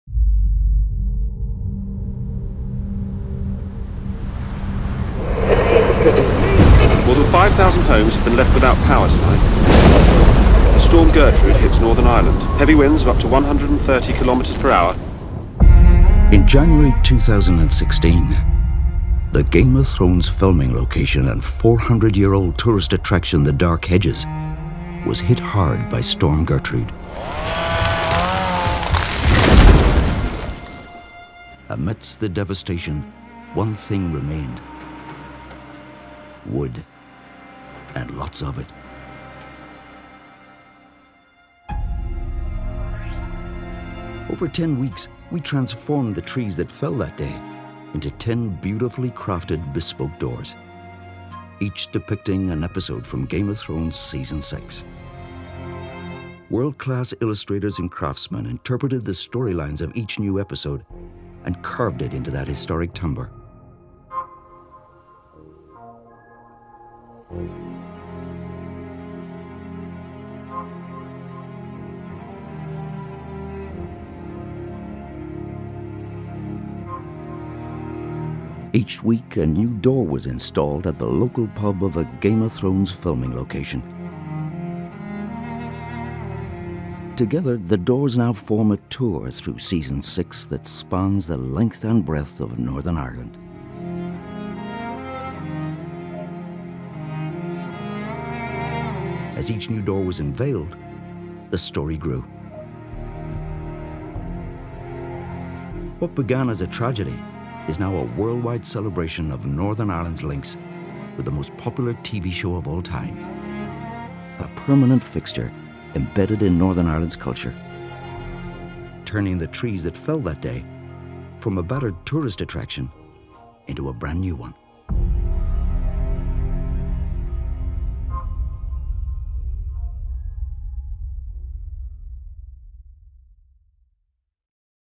Making the Doors of Thrones
When the trees along Northern Ireland's famous Dark Hedges blew over in a storm, we set about saving that piece of Game of Thrones history for fans. We have carved the wood into a set of 10 intricate doors, hung across NorthernIreland, each telling the story of a season 6 episode doorsofthrones. Here's the story of how they were made.